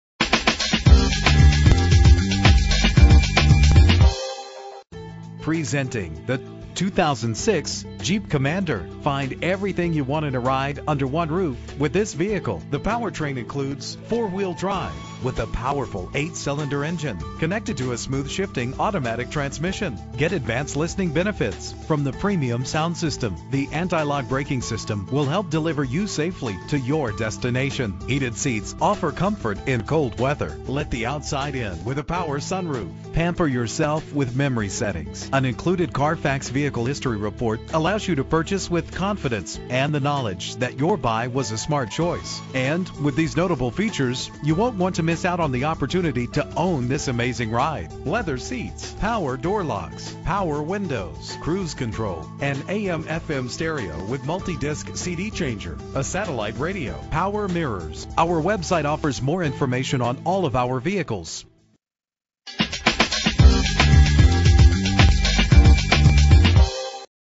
2006 Jeep Commander - Avenel New Jersey
Year: 2006
Make: Jeep
Model: Commander
Trim: Limited
Engine: 5.7 liter 8 cylinder 16 valve
Transmission: Automatic
Color: Gray
Mileage: 63282
Address:
90-100 Route 1 North